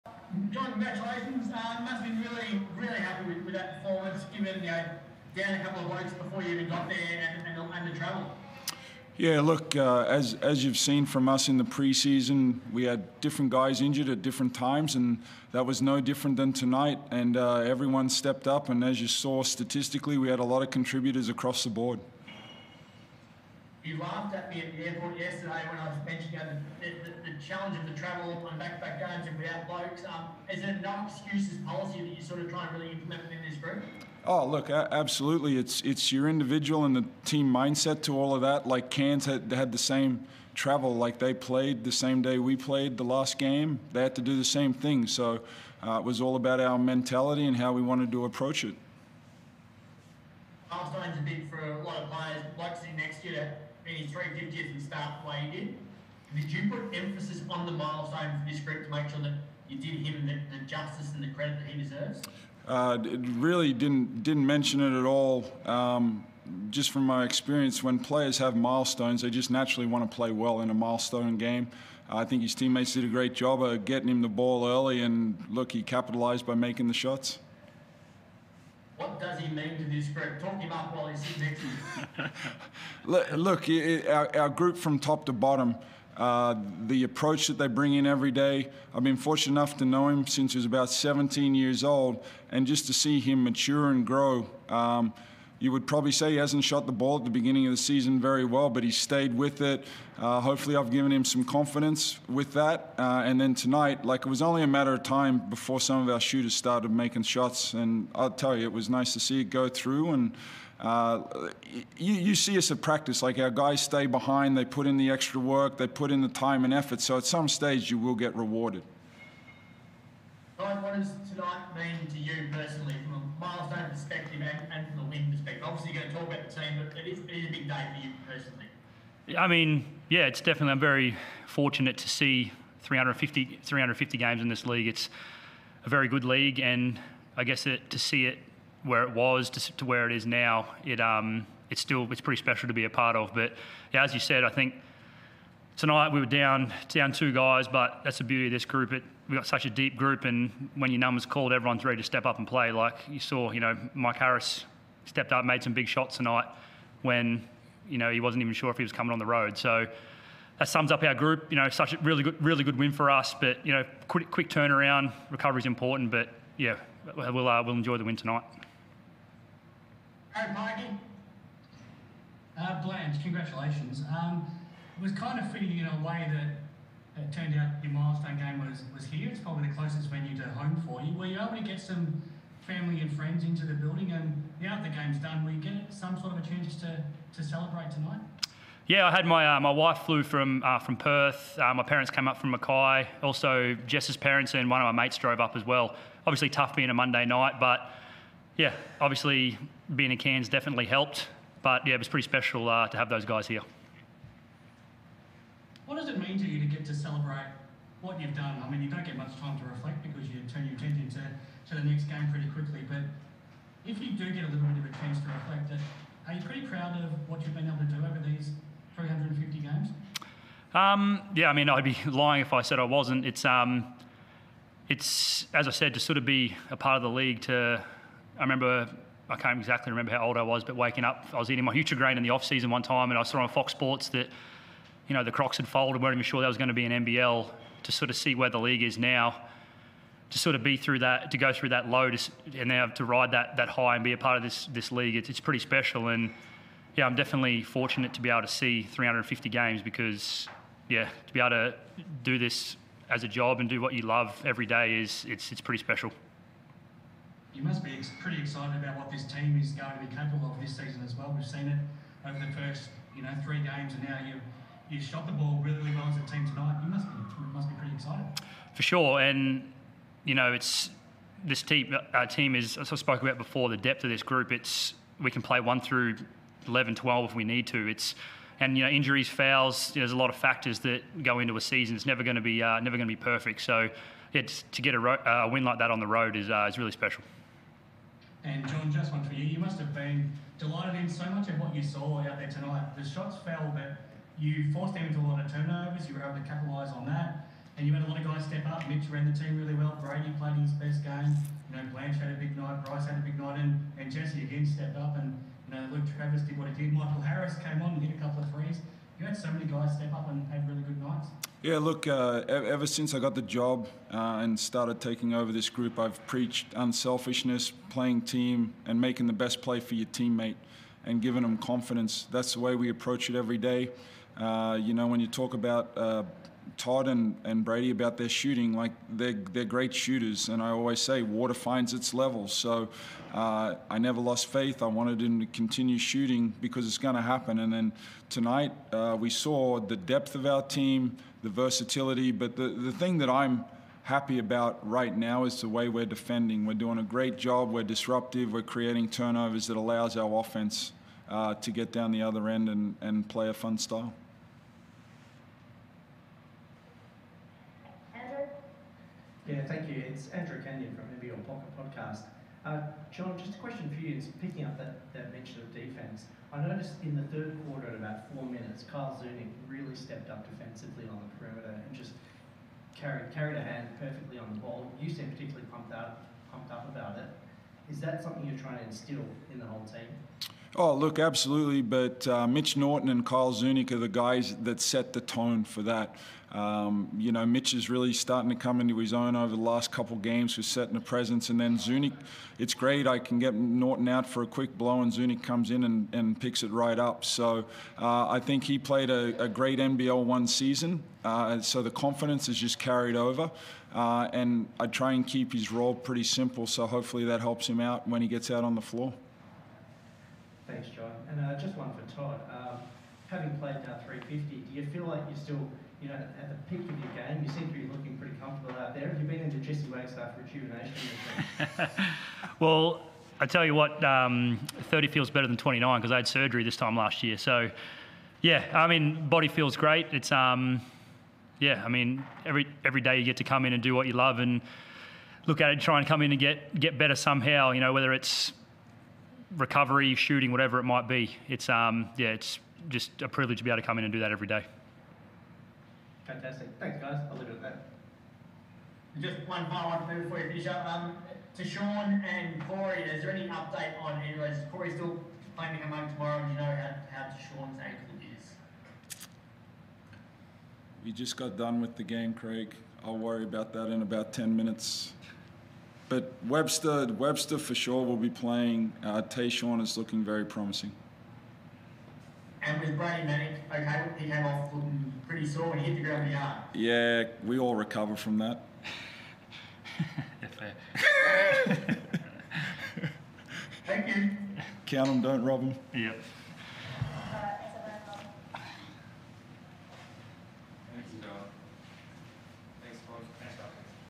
Press Conference - Rillie and Blanchfield vs Taipans
John Rillie and Todd Blanchfield chat to the media after the win and the veteran's 350th game.